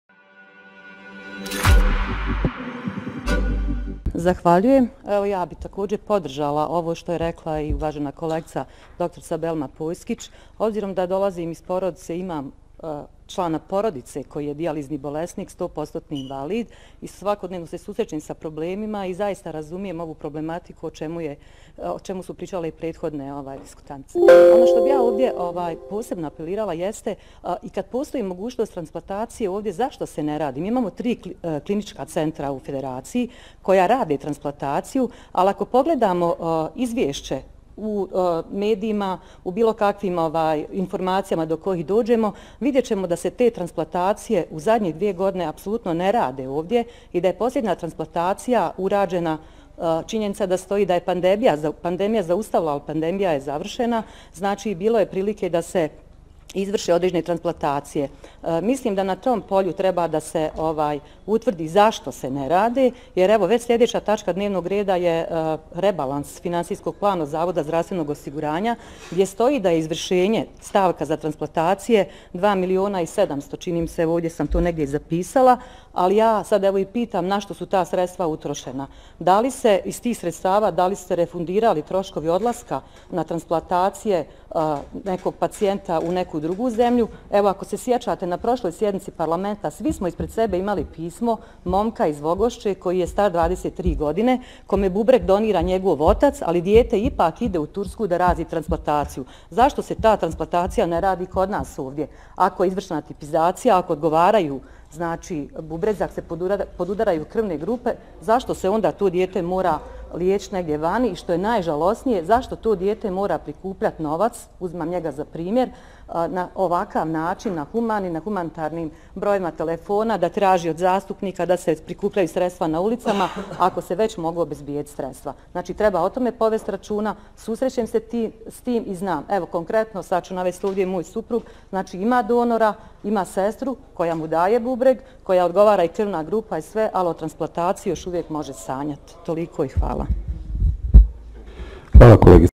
Mujkanović. Na šta su utrošena sredstva predviđena za transplatacije organa?
Zastupnica Mujkanović u svojoj diskusiji se osvrnula na sve mogućnosti u oblasti transplatacije napominjući prisutne da tri klinička centra u FBiH mogu ovu djelatnost da ispunjavaju. Upučuje pitanje nadležnim institucijama,  zašto se transplatacije ne rade u našim klinikama i gdje su utrošena sredstva koja su navedena u Finansijskom planu ZZO FBIH u iznosu od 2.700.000 KM?